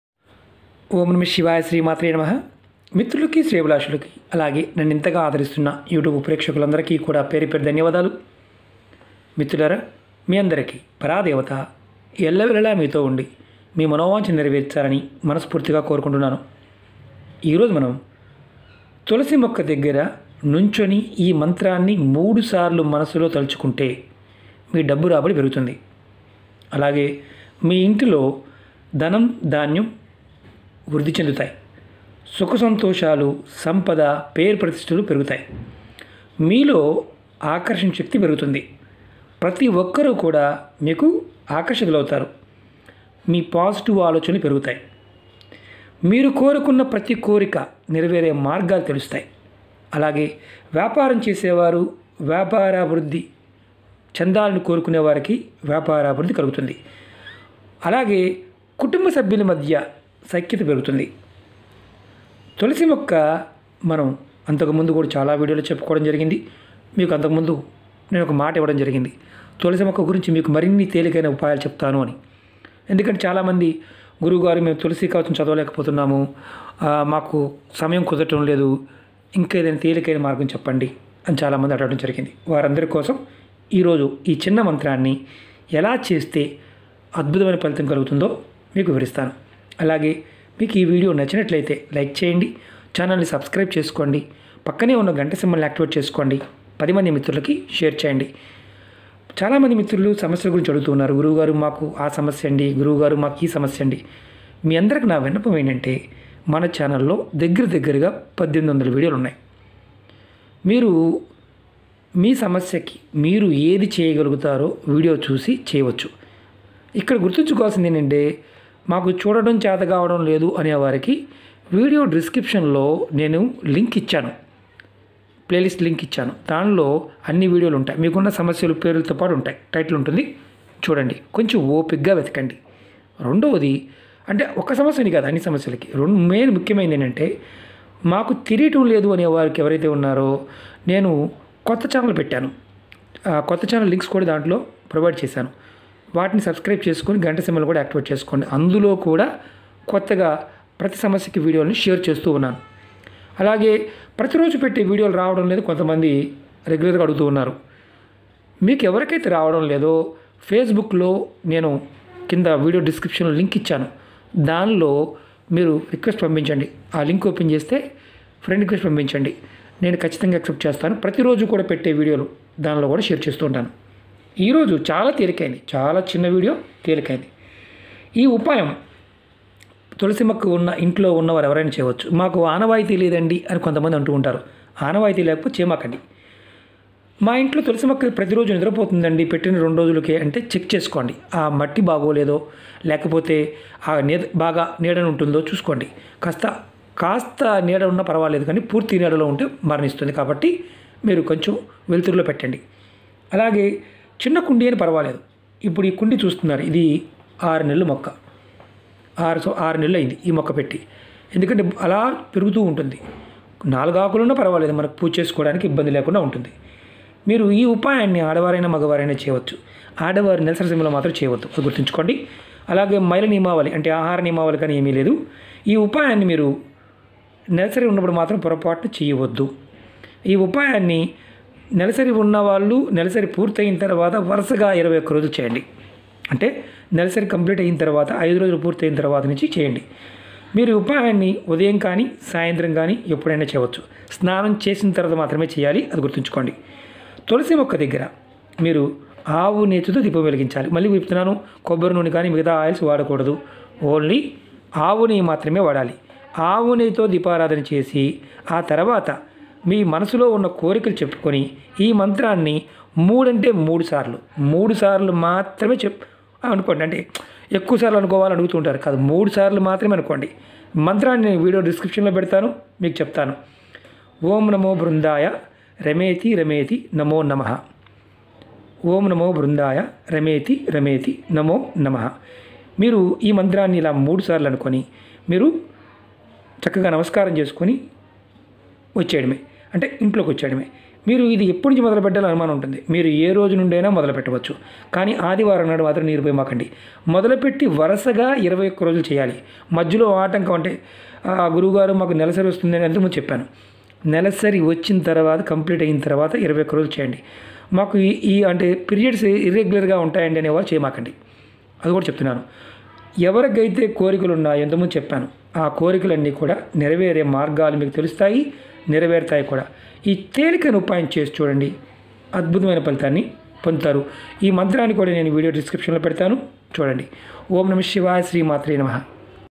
తులసి మొక్క దగ్గర నుంచుని ఈ మంత్రాన్ని మూడు సార్లు మీ మనసులోతలచుకోండి డబ్బు రాబడి పెరుగుతుంది
Time:10.00 AM TO 1.00 PM And 3.00 PM TO 6.00 PM
fees: 500 rupees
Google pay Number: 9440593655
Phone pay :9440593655
ఓం నమో బృందాయ రామేతి రామేతి నమో నమః